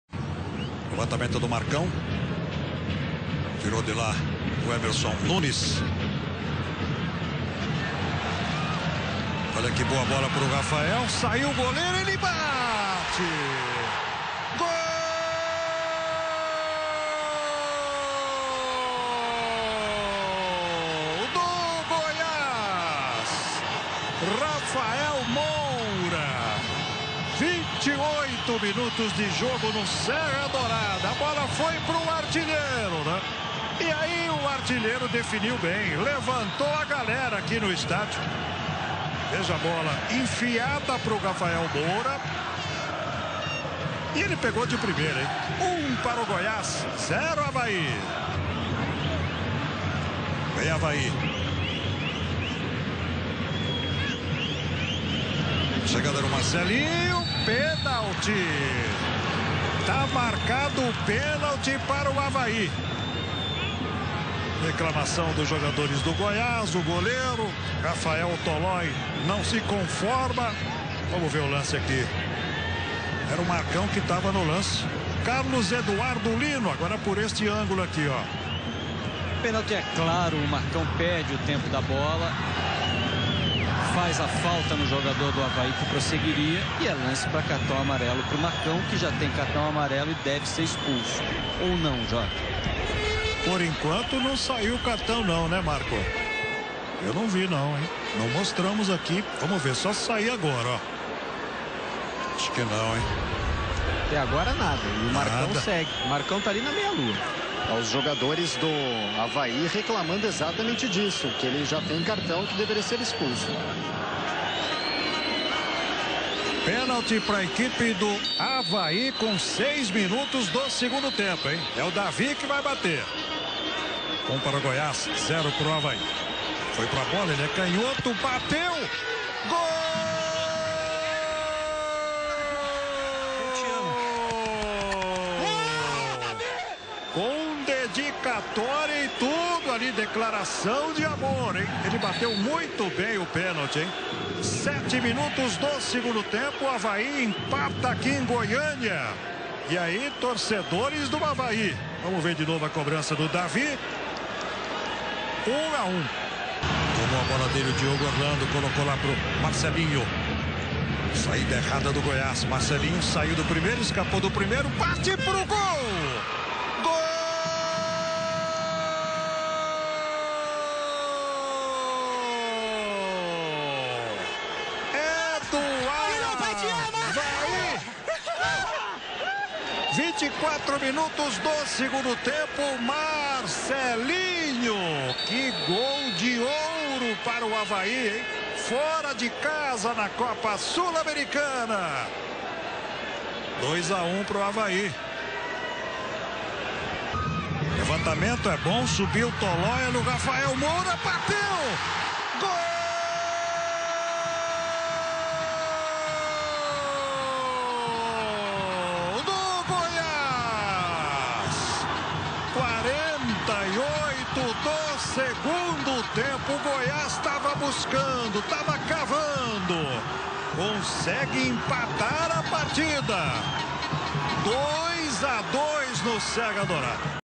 Goiás 2 x 2 Avaí - Copa Sul-Americana 2010
Copa Sul-Americana 2010 - Quartas/Ida - Estádio Serra Dourada - Goiás 2 x 2 Avaí - Gols do Verdão: Rafael Moura (2x) - Público Pagante: 3.742 - Renda: 41.505,00. Imagens: SporTV